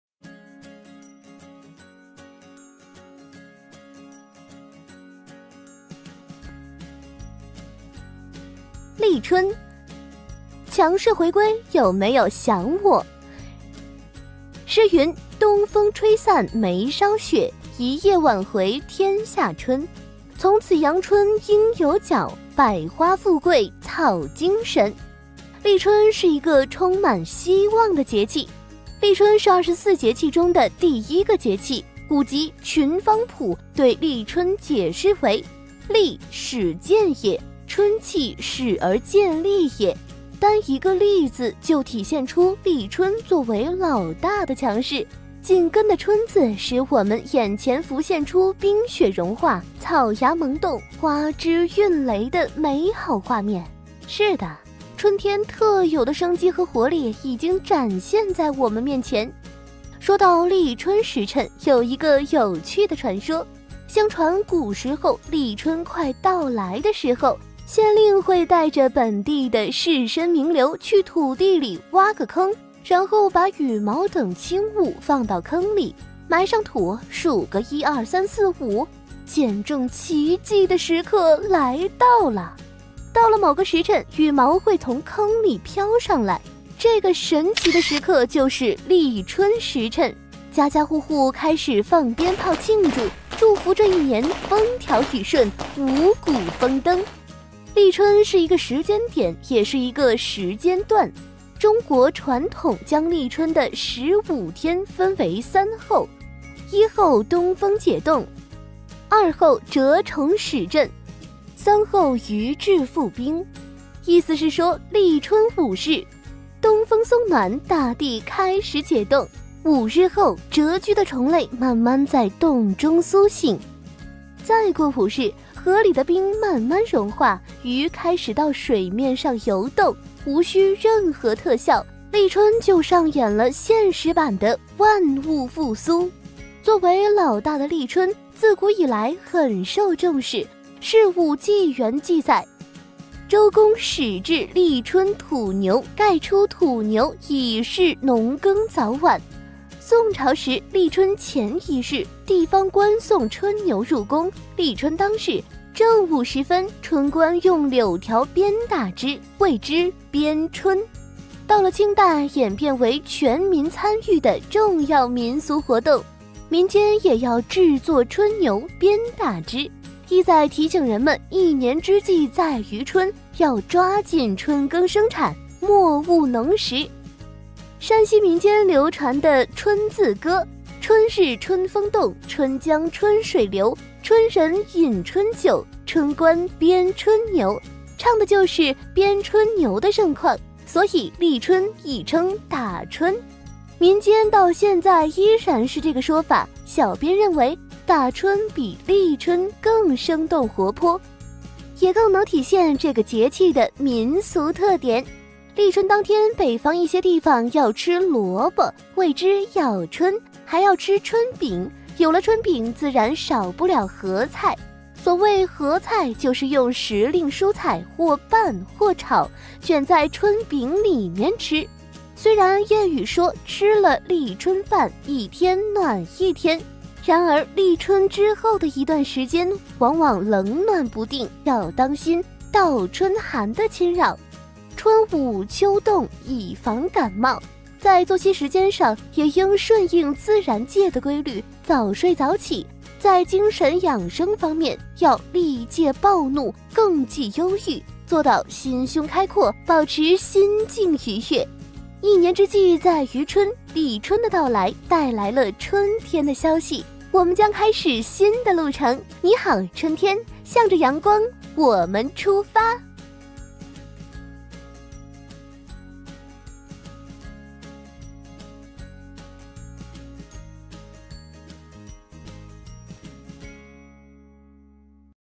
《二十四节气》第1节 立春【学国学网】
学国学网｜专注优秀传统文化教育与传播

节目简介：立春是二十四节气中的第一个节气。古籍《群芳谱》对立春解释为：“立，始建也。春气始而建立也。”一年之计在于春。立春的到来，带来了春天的消息，使我们眼前浮现出冰雪融化、草芽萌动、花枝孕蕾的美好画面。是的，春天特有的生机和活力已经展现在我们面前。